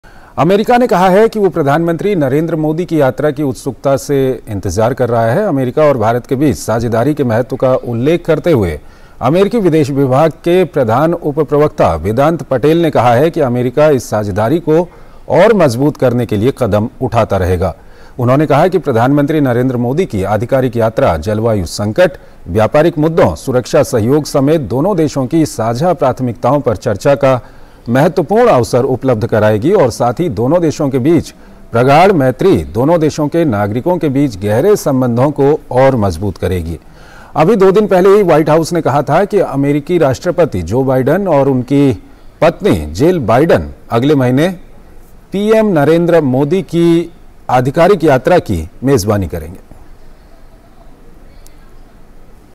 अमेरिका को पीएम मोदी की यात्रा का उत्सुकता से इंतजार
अमेरिकी-ब्रिटेन विभाग के प्रधान-उप प्रवक्ता वेदांत पटेल ने बताया कि अमेरिका को पीएम मोदी की यात्रा का उत्सुकता से इंतजार है